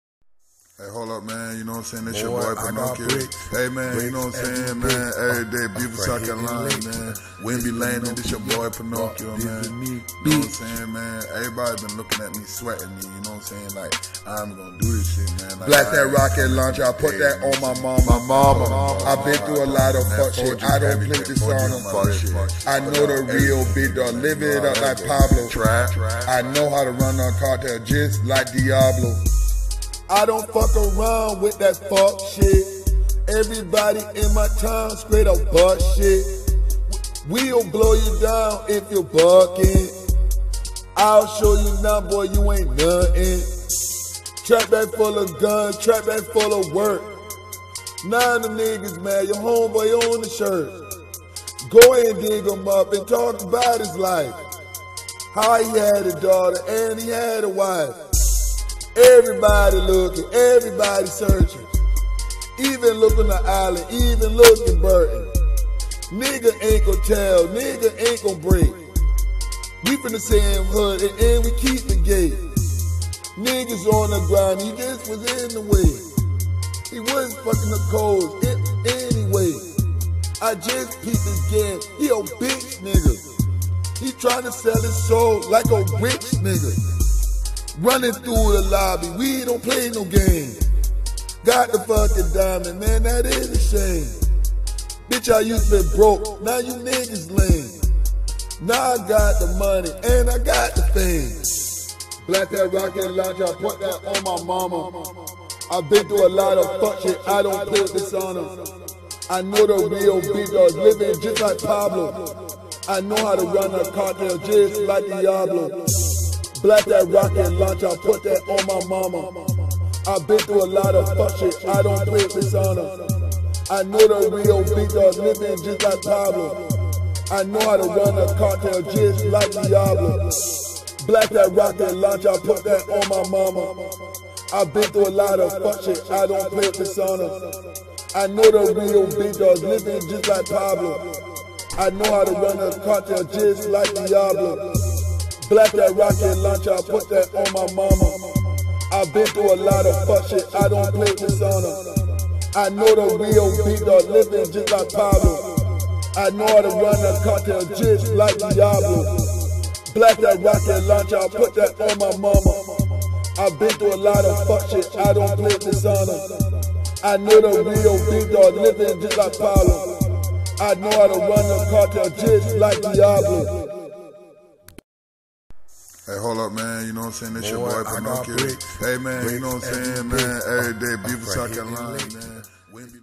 Pinocchio The Real Boy 7x7=49
4g Pinocchio I'm from South Carolina niggas playing with me taking my shit so now I'm taking they shit oh yeah fuck offset for doing Nikki like that his bitch ass lil pump ugly ass stealing my shit my old style bitch ass niggas don't got shit to do stealing my shit yaughty stole from me so I'm stealing from them